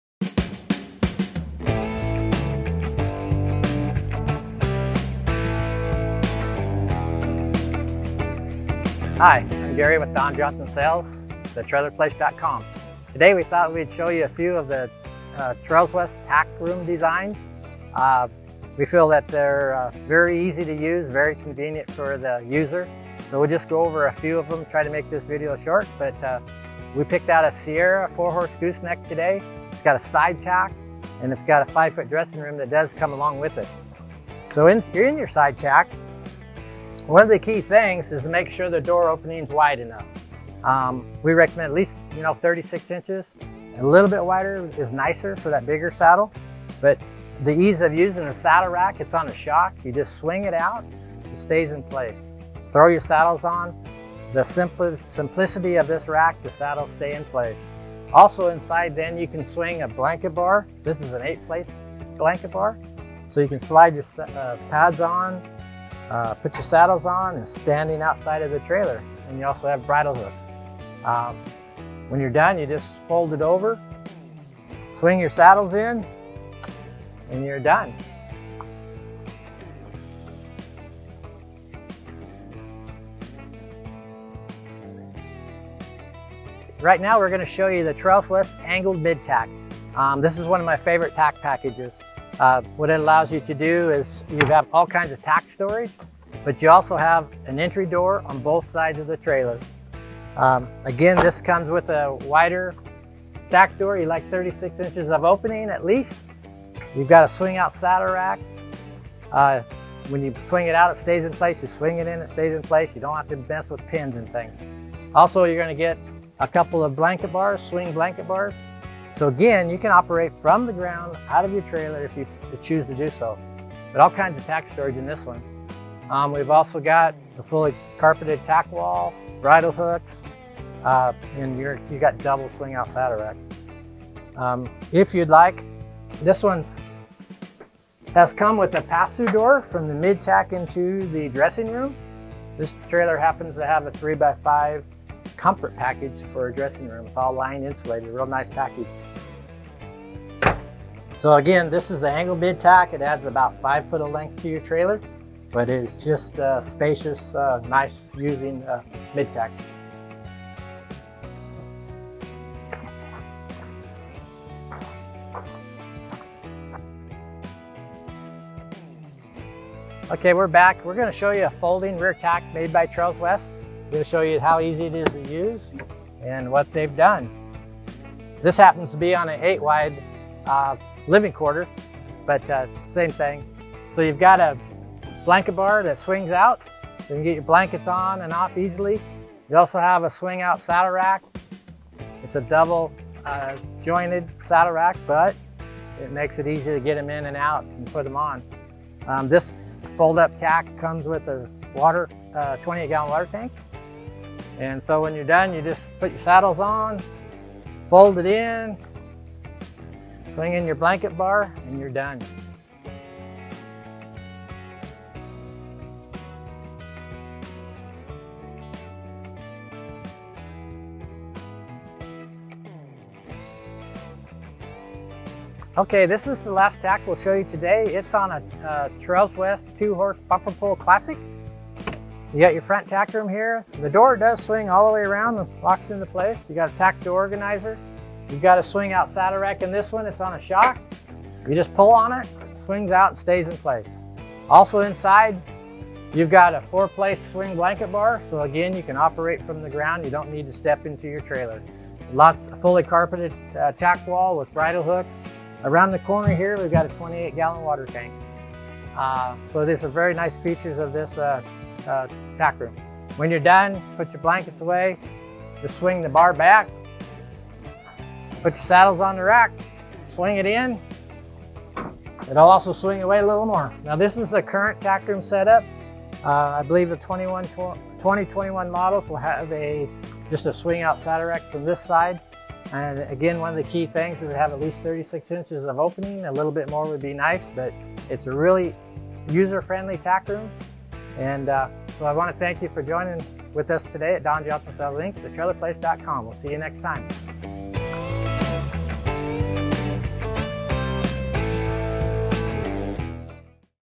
Walk through of Trails West Tack Rooms
Gary of Don Johnson Sales, Inc. walks through various Trails West tack room configurations and shows several placements of swinging saddle racks and blanket bars.